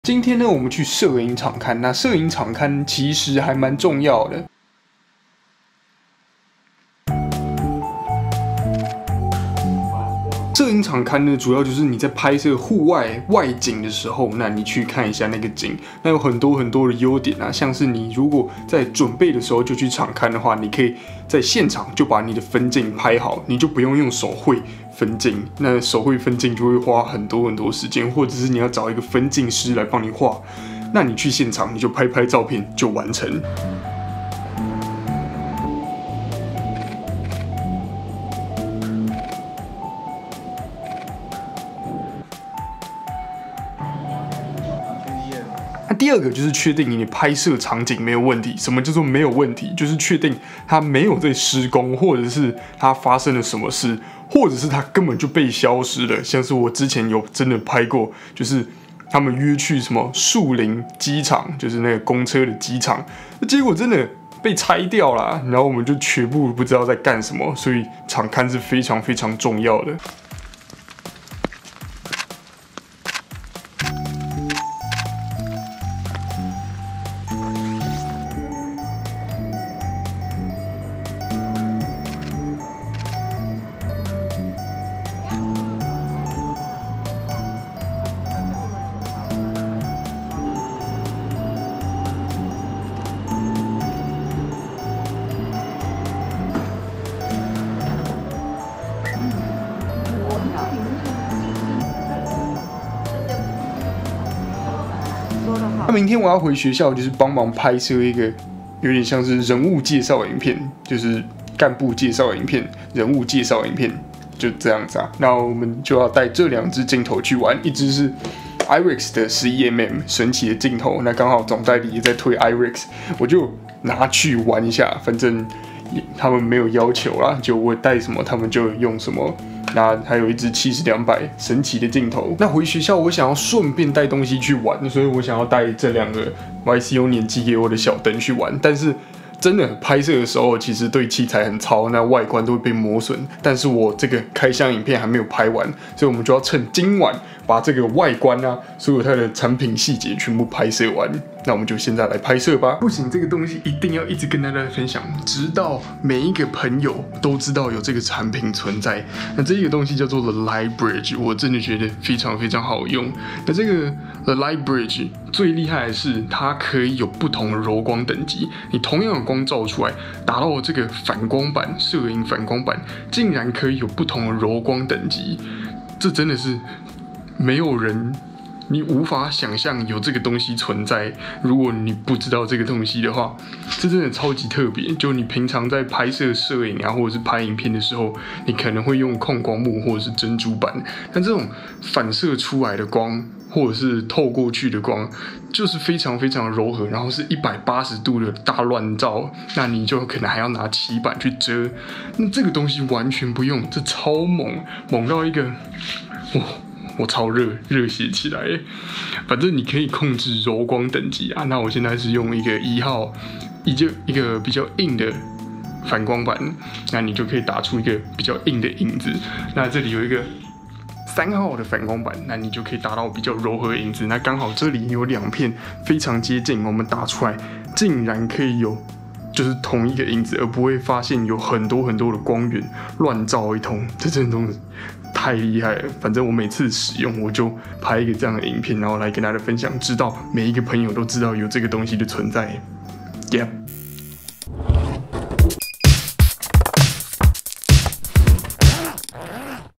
山海逛逛 攝影場刊的重要! 拍攝前期工作規劃 分鏡製作 影視廣告拍攝
拍攝前期工作規劃 分鏡製作 影視廣告拍攝
山海逛逛 攝影場刊的重要! 不然就會出事~ .